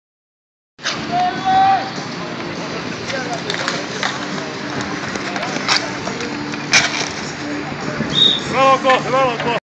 AHK 2011: Veskari torjuu Krisun one-timerin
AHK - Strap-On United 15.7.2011